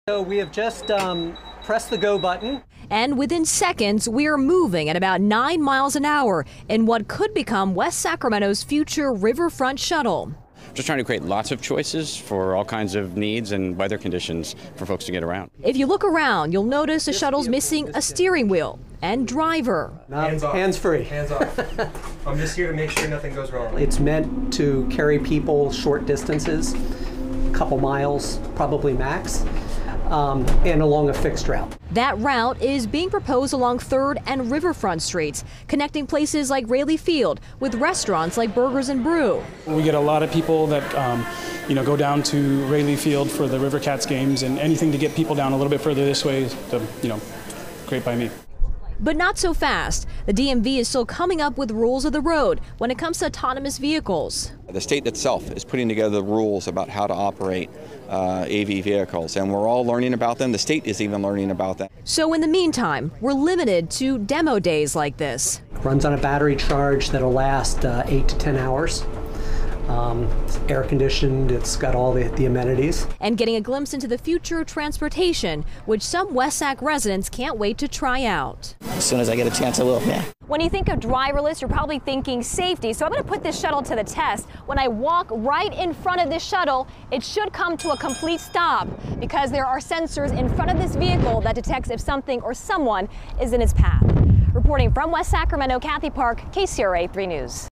West Sacramento’s futuristic shuttle doesn’t need a driver
The future of travel may be coming to West Sacramento. Officials showed off a driverless shuttle that could soon be in service around the waterfront.